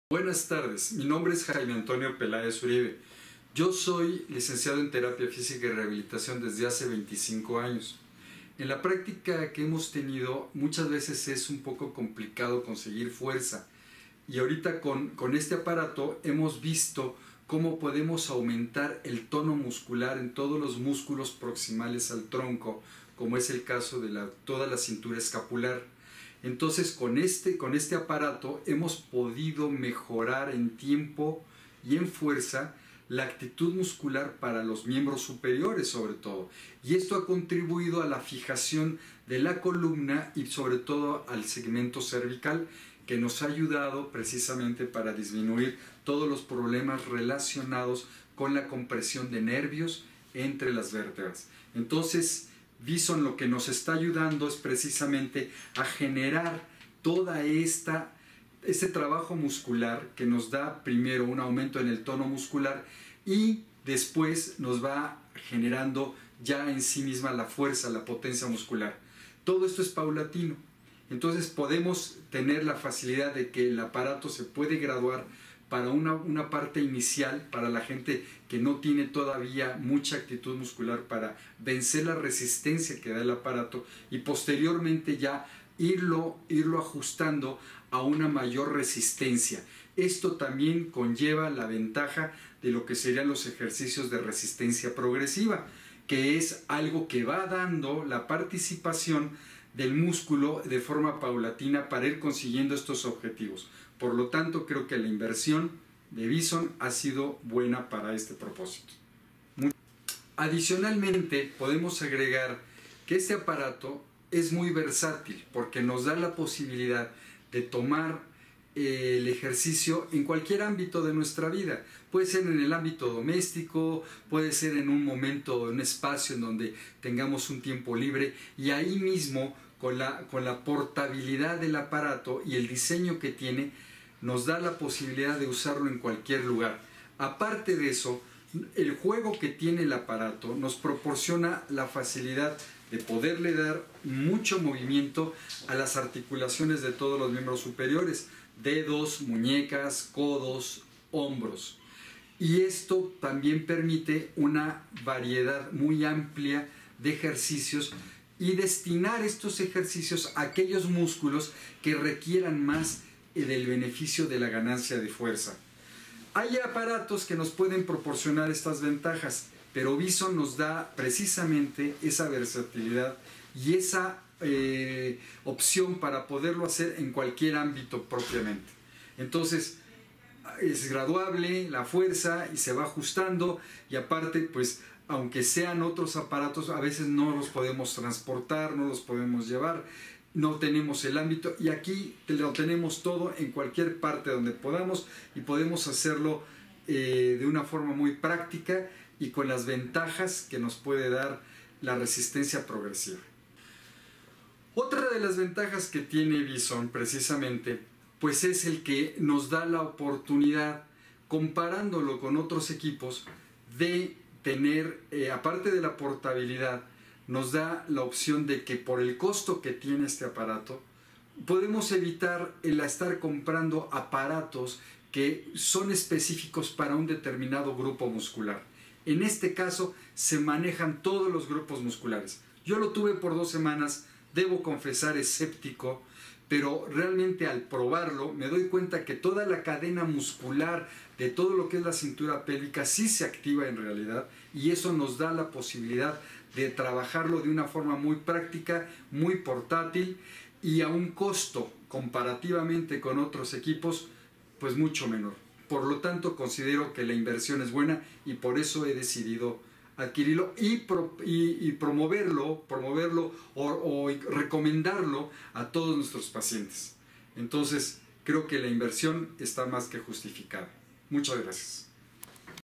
Rehabilitación de los brazos Ejercitador BISON testimonio del fisioterapeuta Jaime Pelaes
Llámanos sin costo: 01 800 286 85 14

Vital Spark S.A. de C.V. Distribuidor exclusivo de Bison en América Latina.
Tuxpan#2 Interior 402 Colonia Roma Sur, Delegación Cuauhtémoc, México D.F.